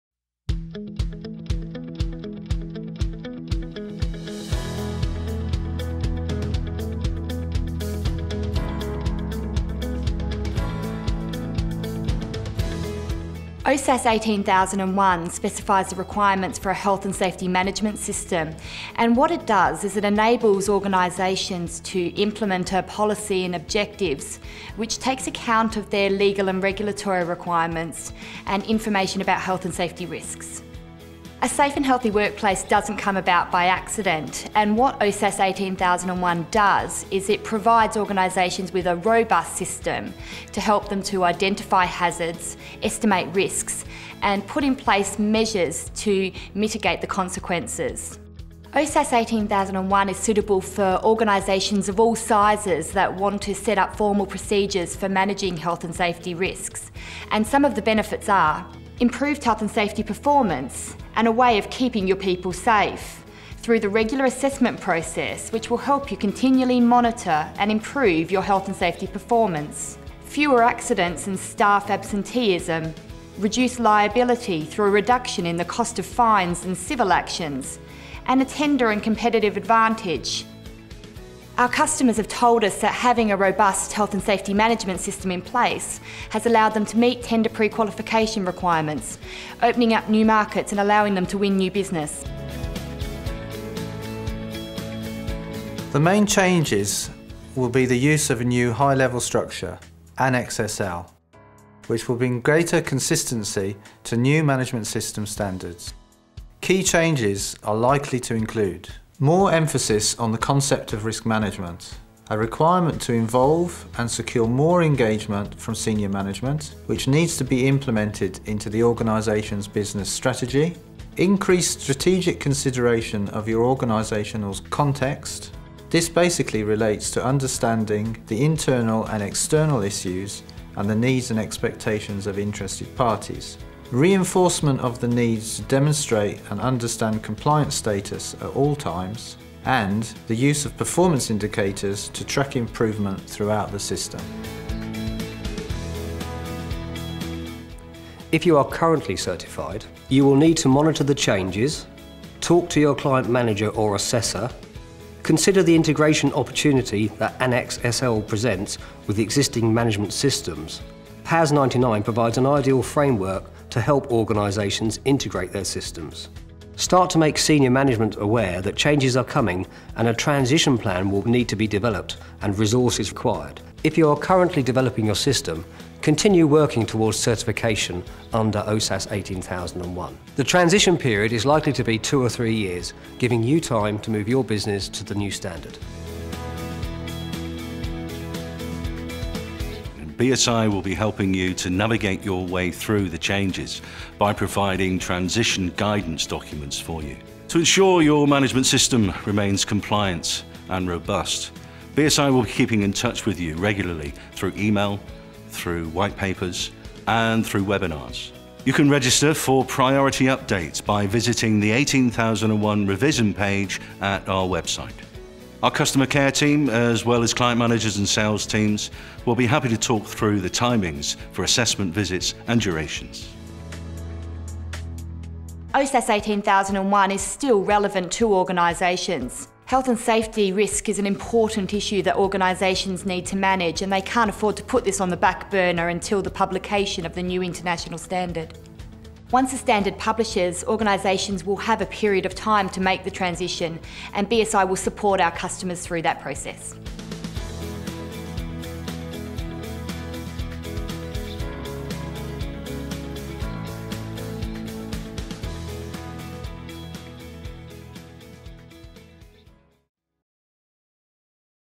Making the migration from BS OHSAS 18001 to ISO/DIS 45001
BSI explains the background of the to moving from BS OHSAS 18001 to ISO 45001 and how clients can manage the transition when the new standard.